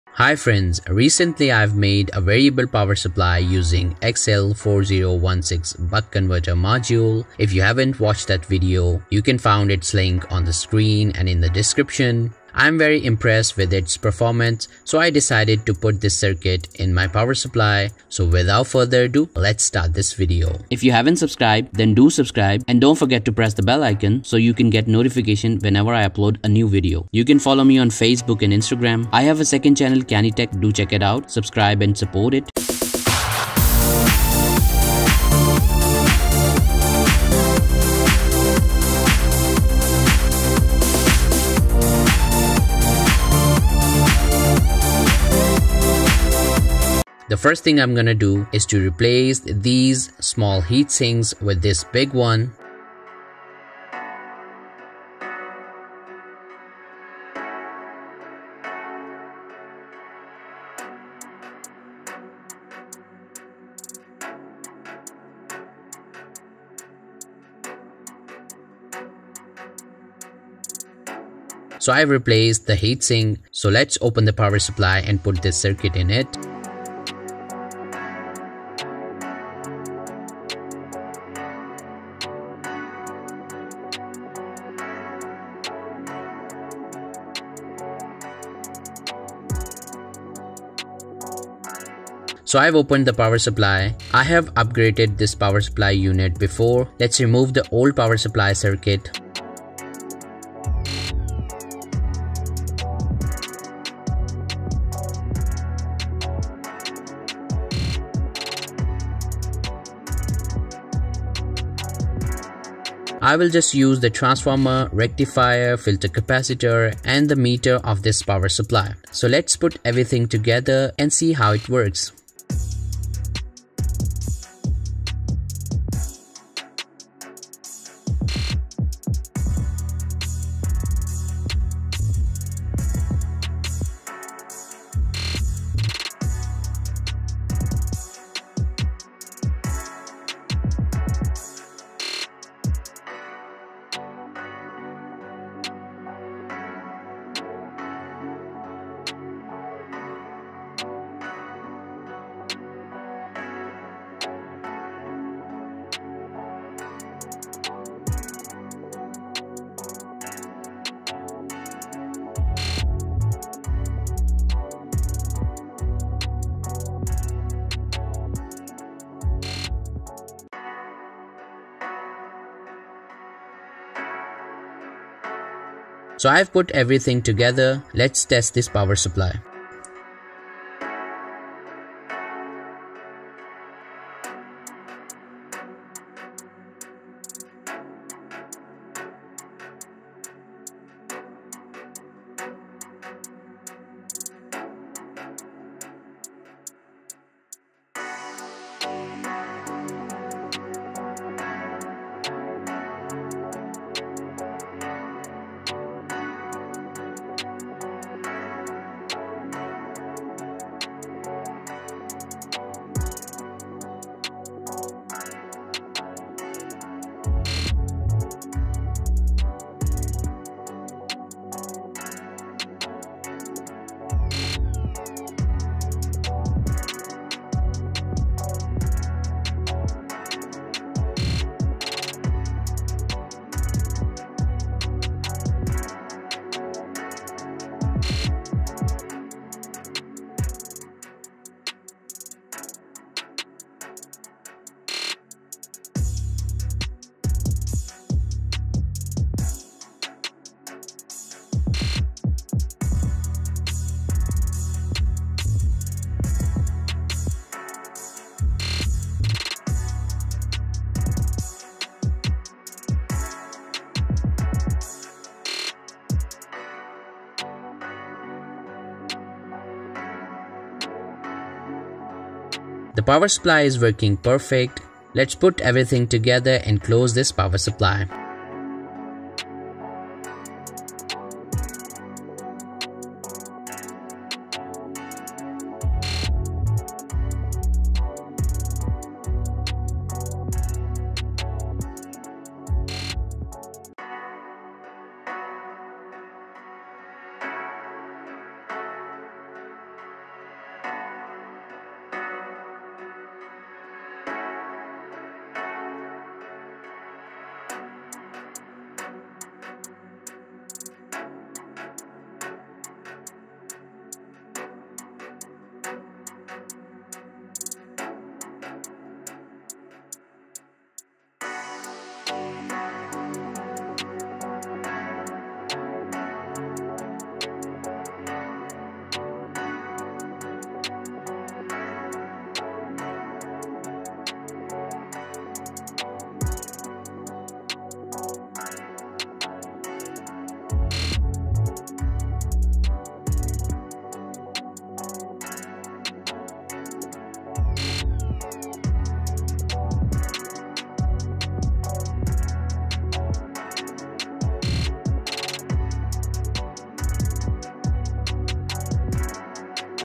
Variable Lab Bench Power Supply Modification
Hi friends in this video i have modify my bench power supply which was a linear power supply circuit which works good but dissipates little more power so i replaced the power supply circuit with xl4016 module buck module now the power supply is working flawlessly hope this will help you please like share and subscribe

xl4016 module

lm2596 module

volt amp meter

10k multi turn potentiometer

silver blak knob